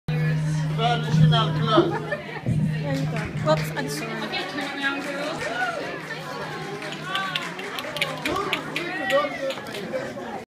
Ethiopian dresses
Short video of yesterday's Heritage Day at the school. My student is the one on the left.